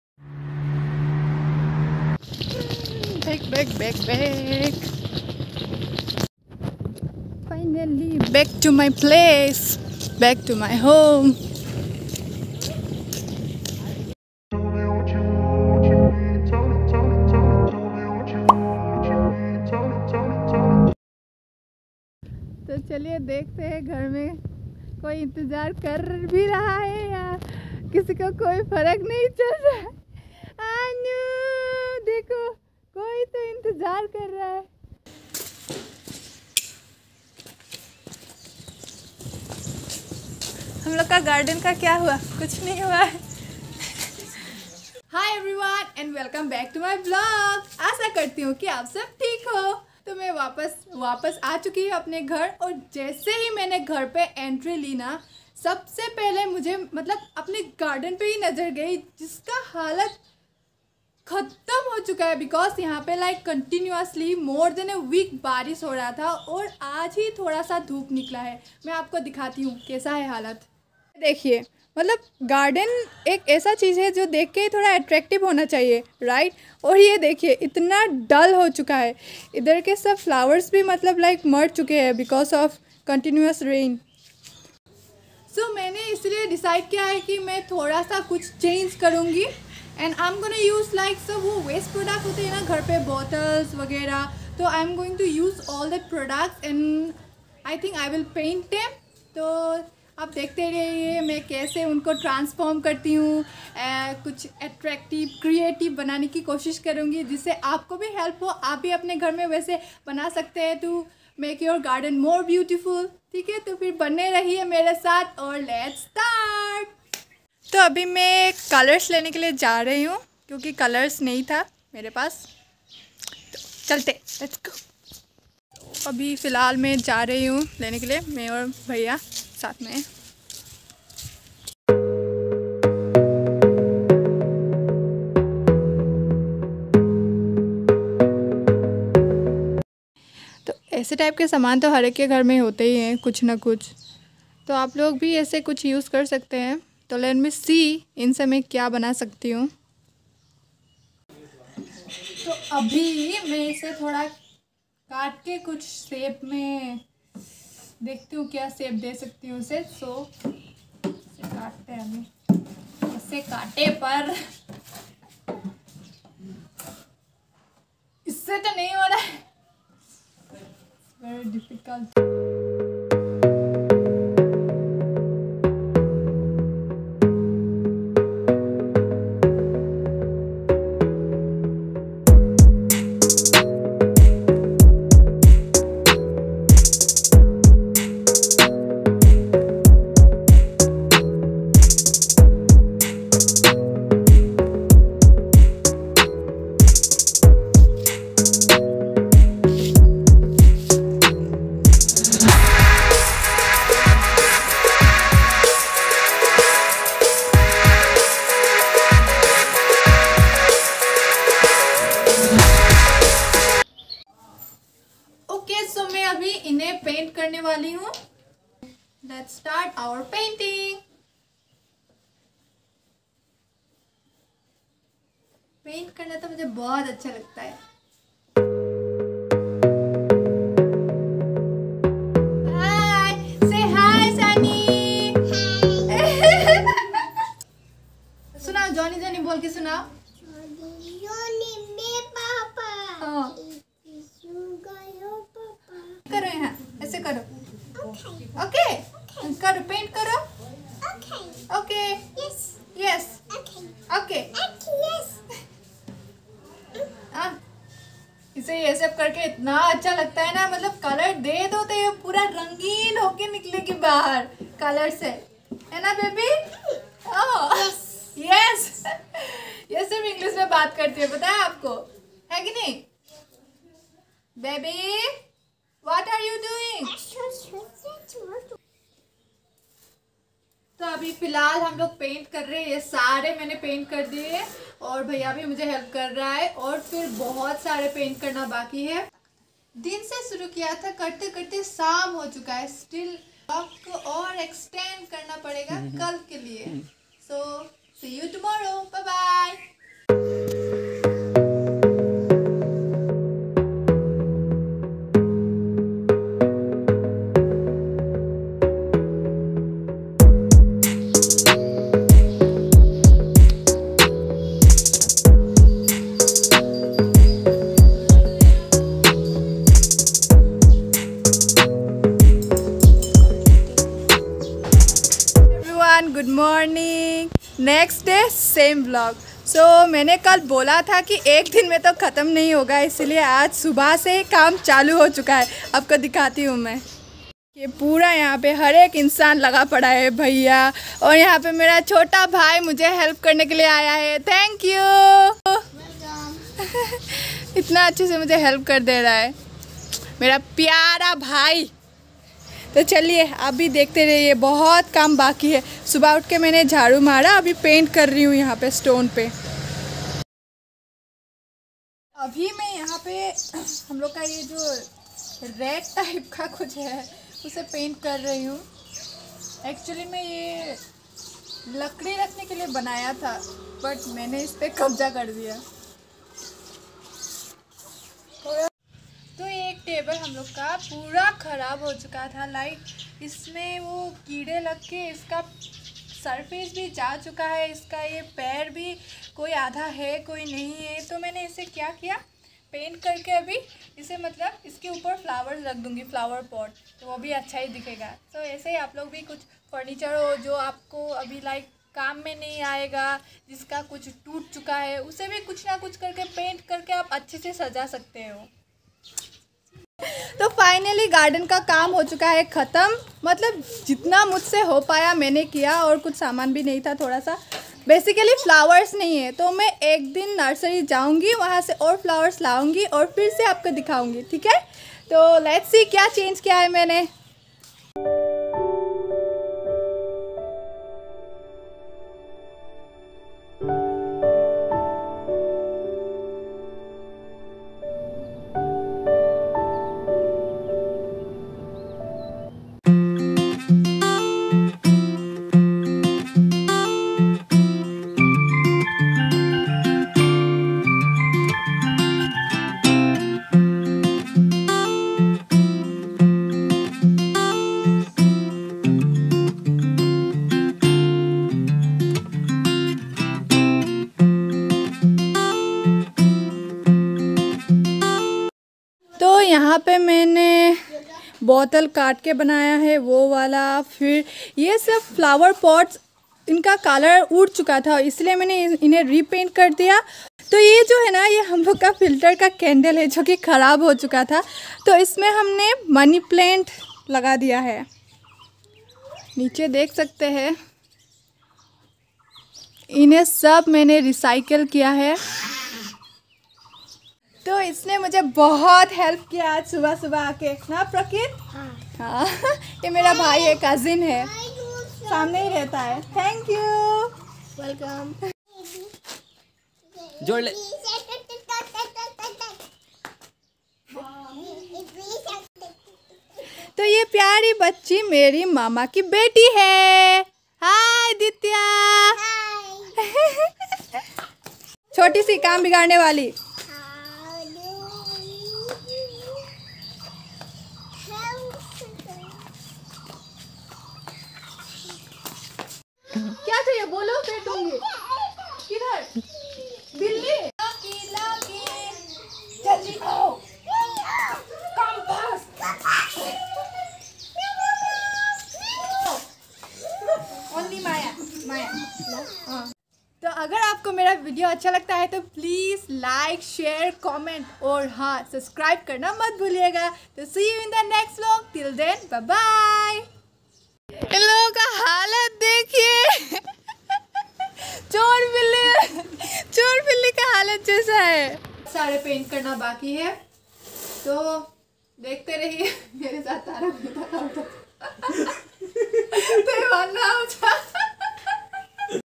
DIY Garden hacks||vlog||back to home vlog
Back to my hometown Sadiya. Improving my garden using DIY hacks. Easy way to decorate our garden using colours, unwanted furniture, plastic bottles,oil cans,etc.